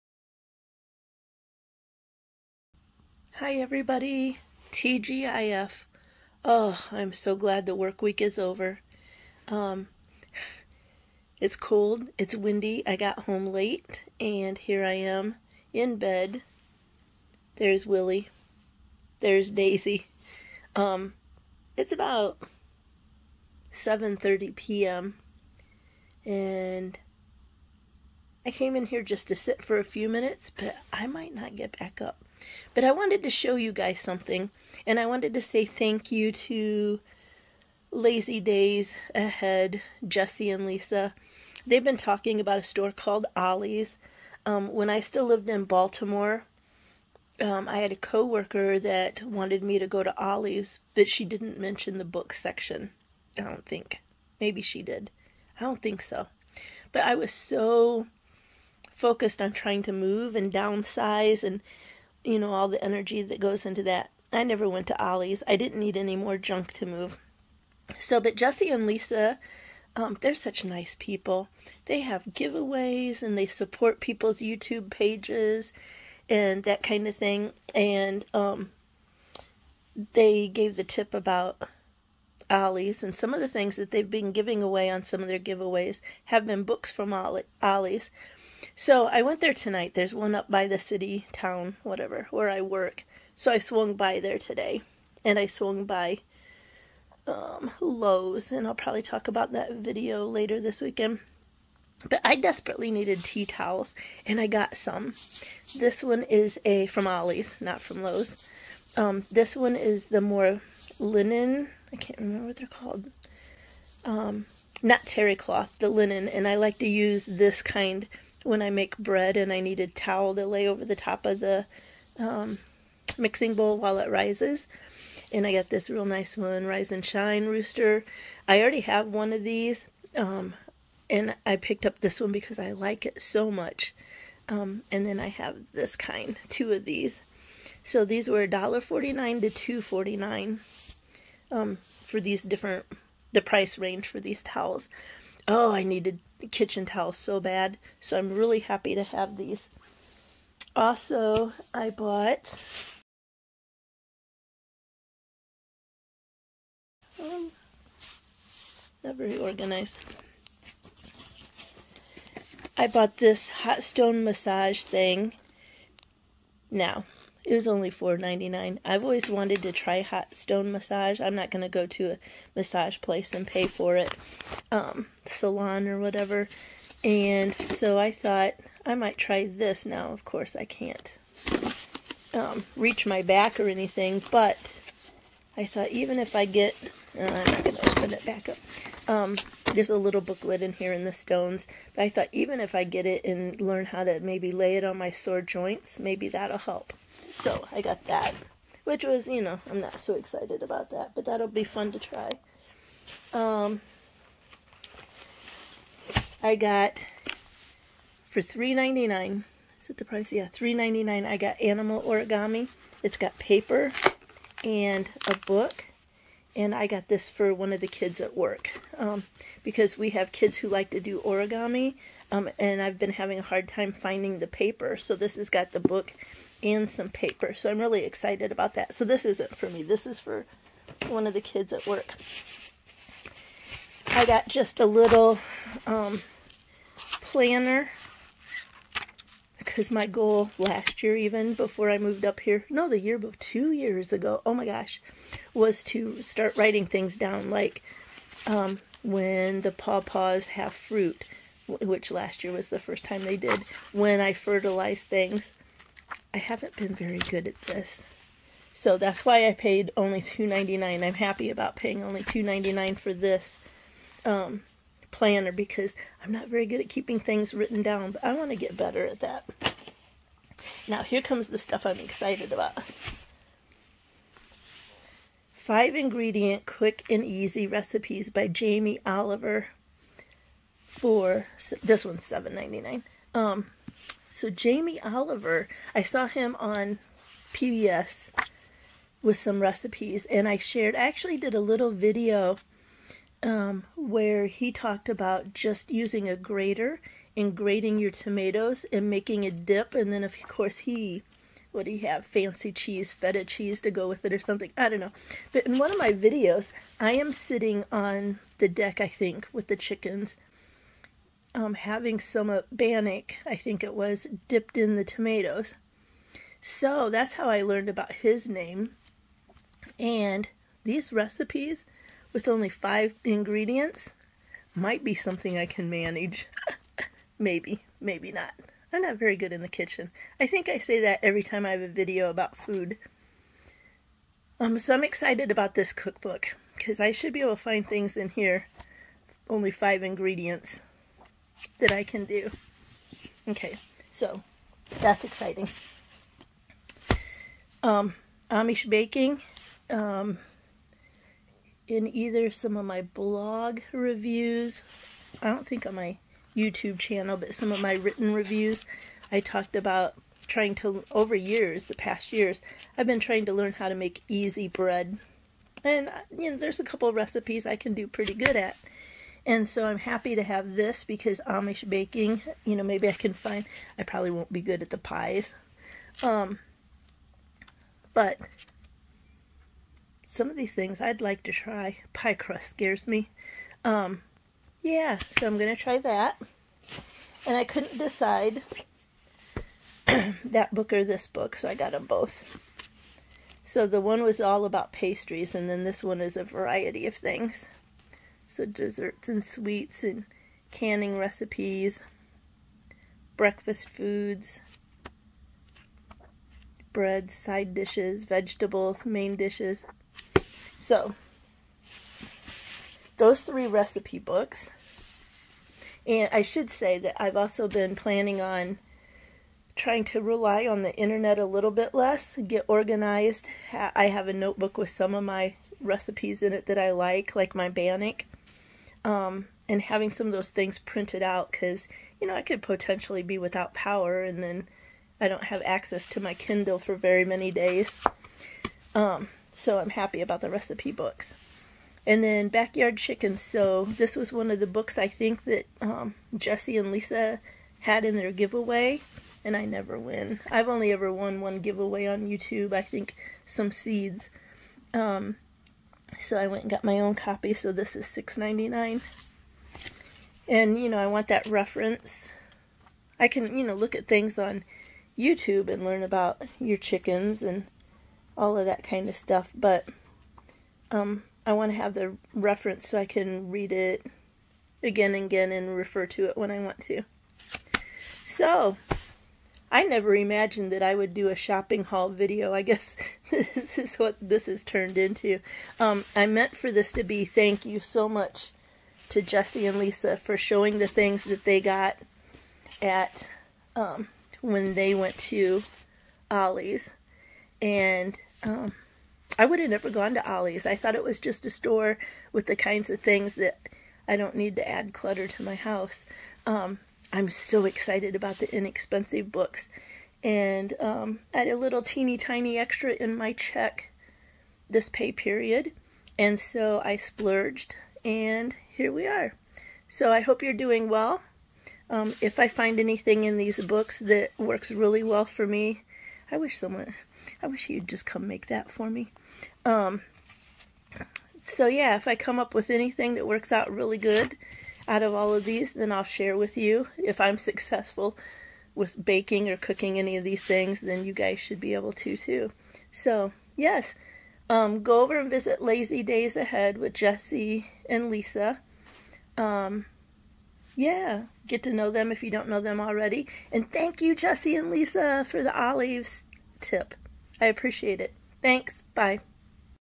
Ollie's Haul - Thank you
Thank you Jesse and Lisa @LAZYDAYZAHEAD for  talking about Ollie's and sharing the things you purchased there. I was surprised by a couple extra bucks in my paycheck and headed to Ollie's after work tonight.

July video... sitting on the deck with chickens grated fresh tomatoes and bannock.

My Mailing Address:
Dawn Rae